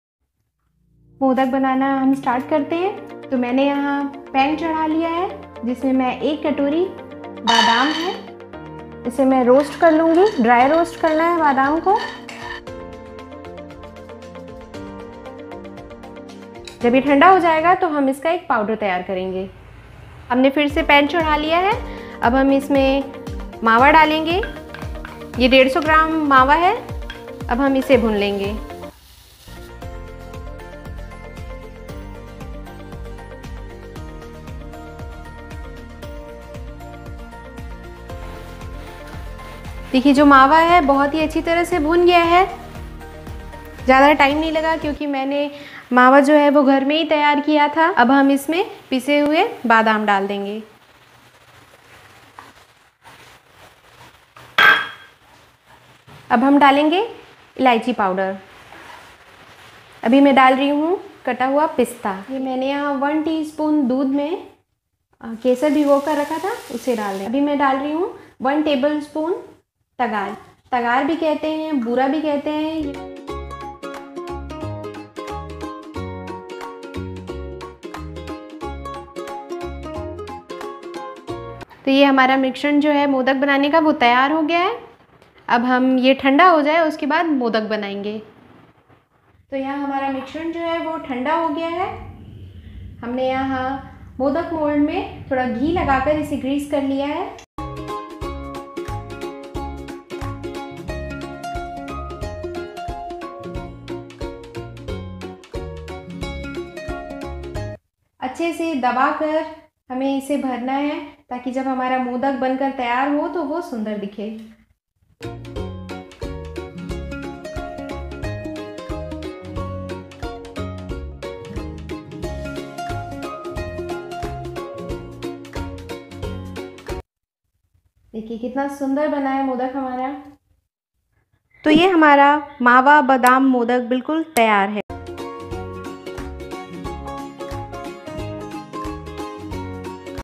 मोदक/गणेश चतुर्थी विशेष/ modak recipe special on Ganesh pooja
भगवान श्री गणेश जी का स्वागत करें मोदक से😊